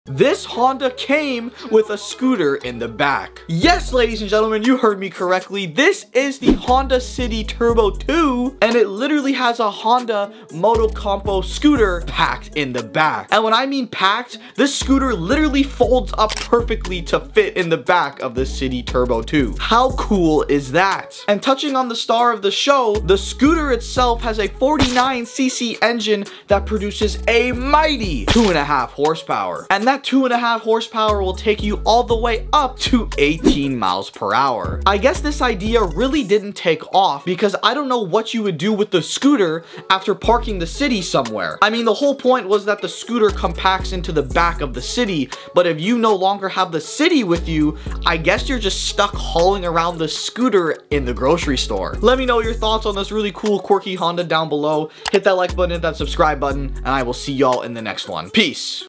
This Honda COMES WITH A SCOOTER?!!
This is a cult classic Honda. The City ii Turbo is amazing, even without the addition of the Motocompo.